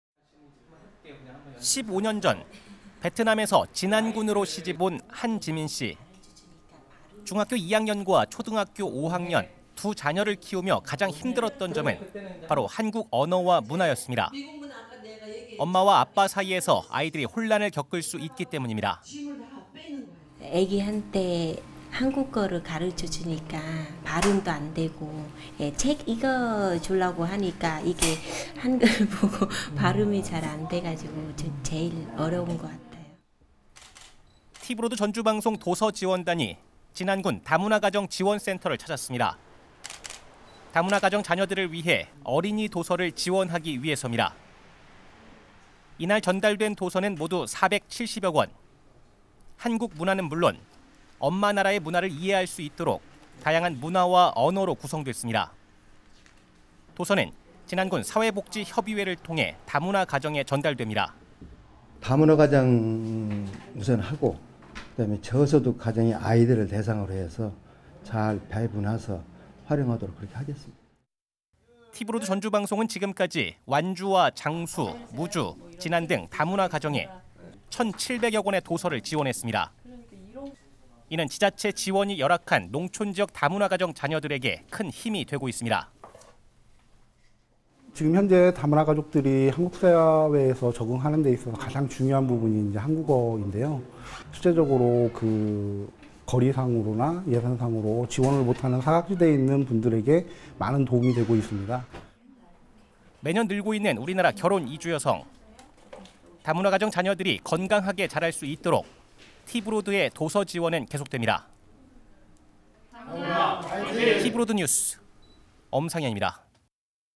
티브로드, 다문화 도서 지원
티브로드가 사회공헌의 일환으로
진안지역 다문화가정 자녀들을 위해
어린이 도서를 지원했습니다.